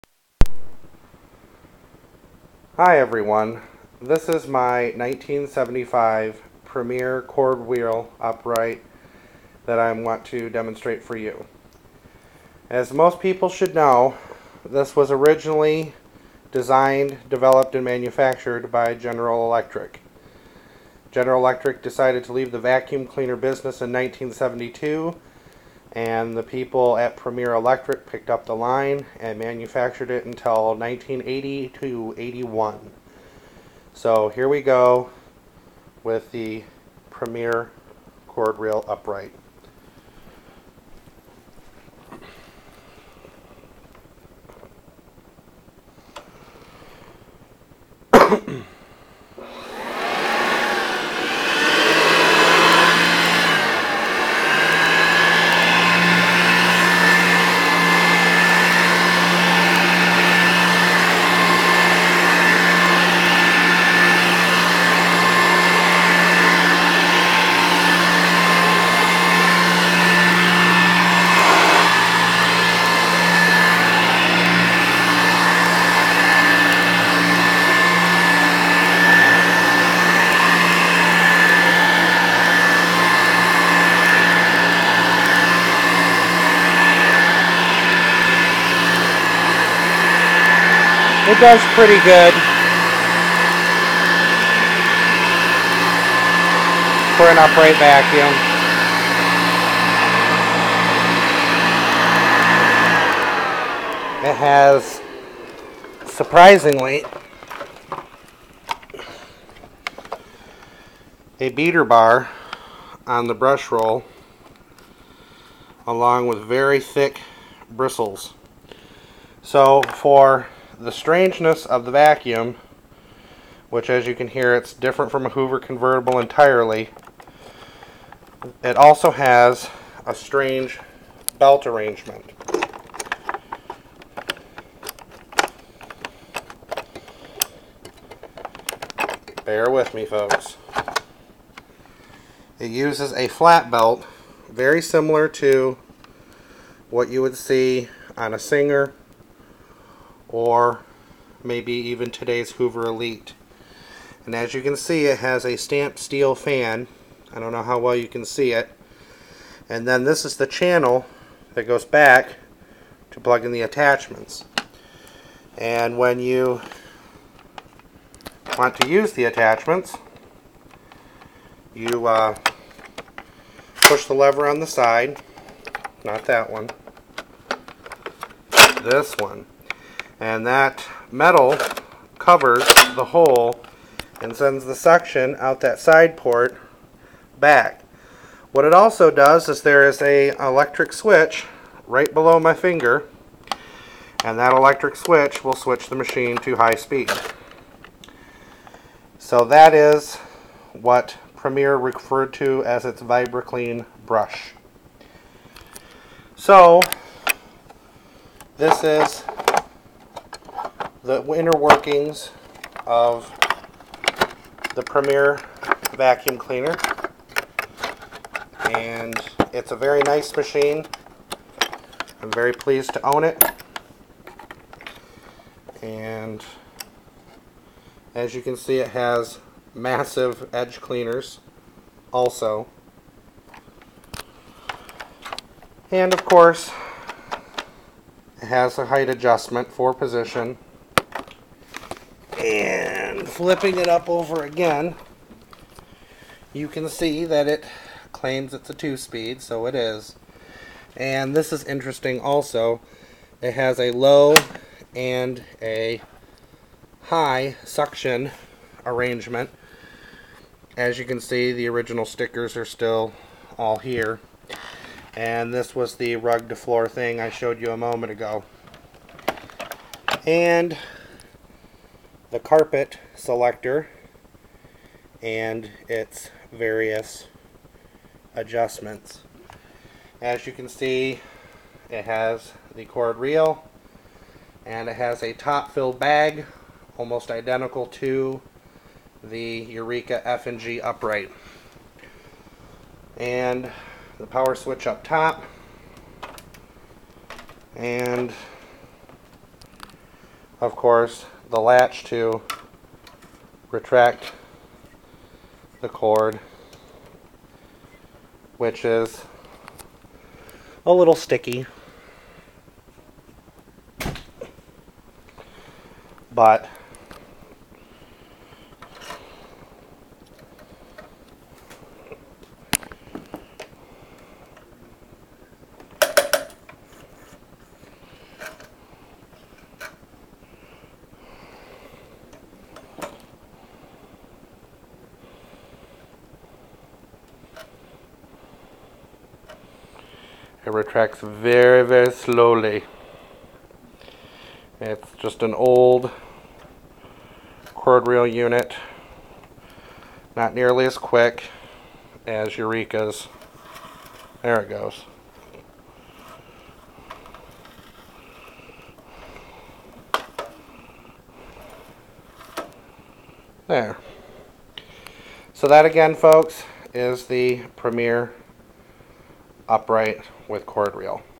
1975 Premier Cord Reel Upright Vacuum Cleaner. Designed by General Electric
I show you all my 1975 Premier Cord Reel Upright Two Speed, as I put it thru a few paces on the living room rug.  This machine was a gift from Kyle Krichbaum.